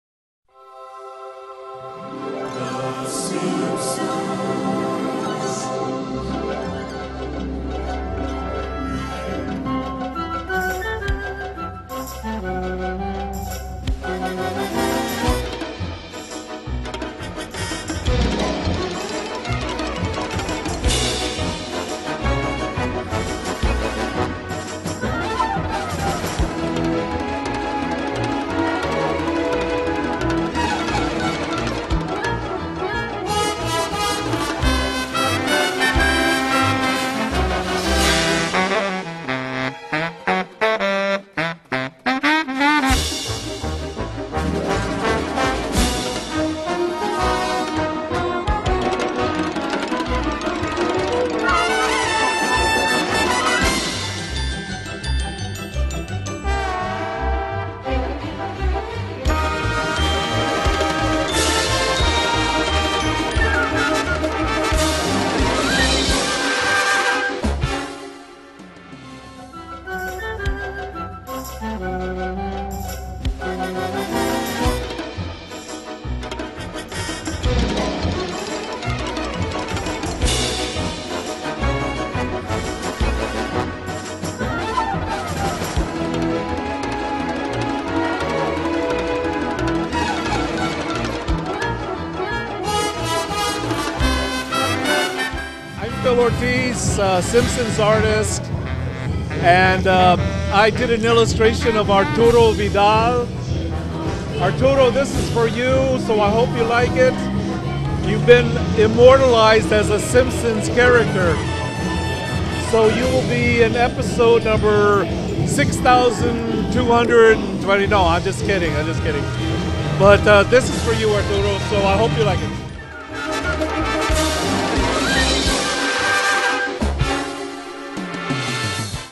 Así sería Arturo Vidal si fuera personaje de Los Simpsons
La Cuarta fue a la Expo Cool y se paletió con que Phil Ortiz, dibujante original de la serie, "simpsonizara" a Celia Punk.